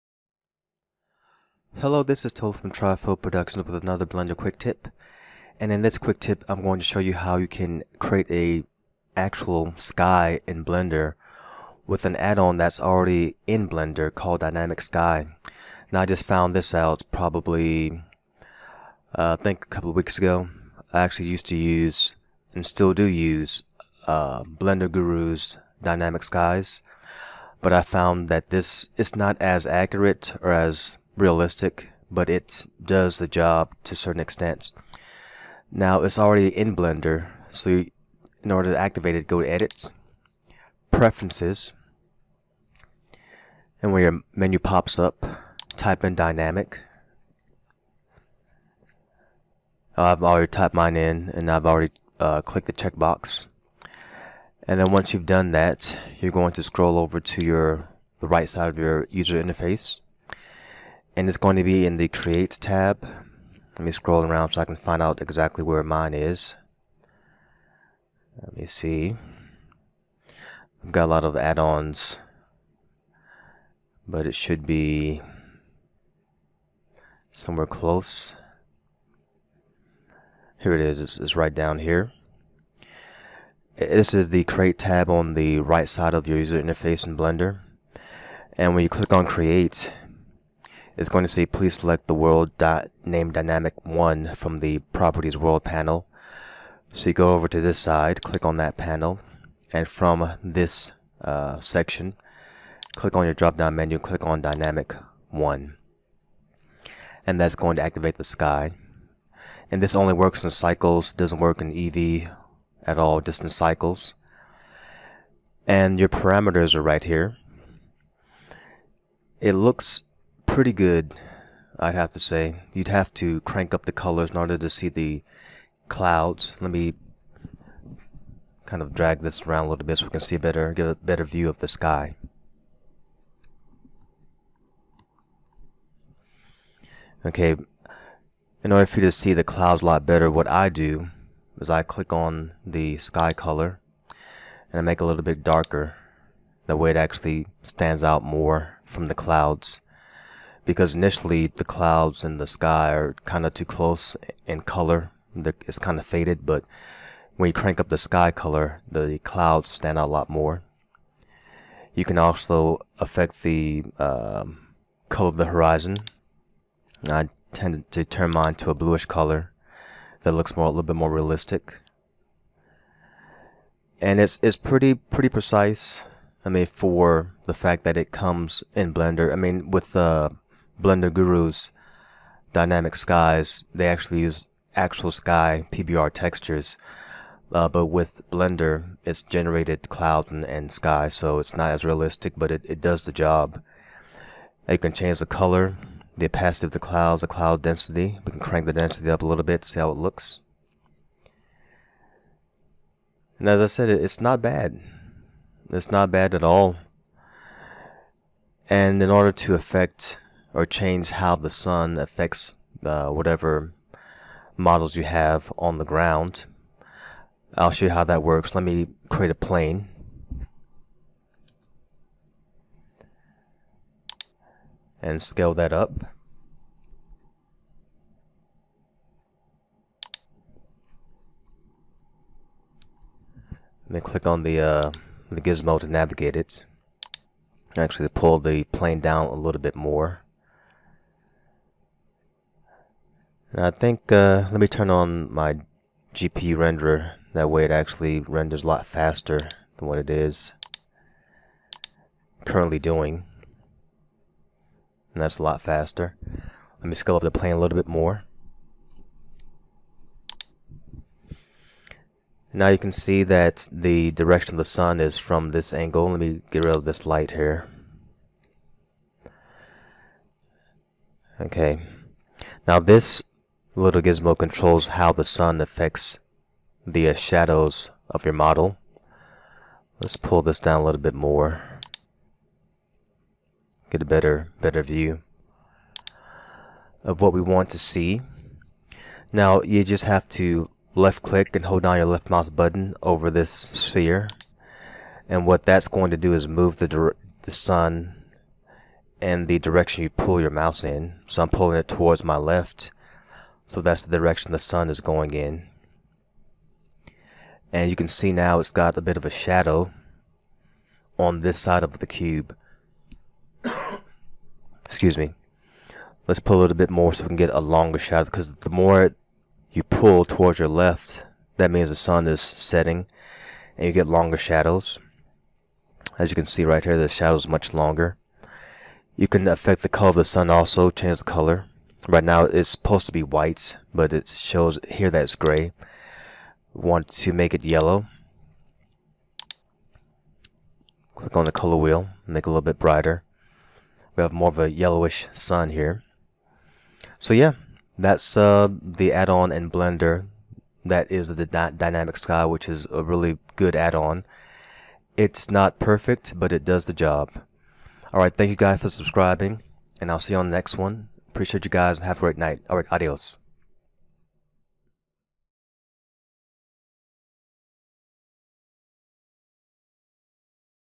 Blender Quick Tip: Create a sky using Blender's Dynamic Sky Addon
In this tutorial you'll learn how to create a sky the easy way using Blender's built in dynamic sky addon.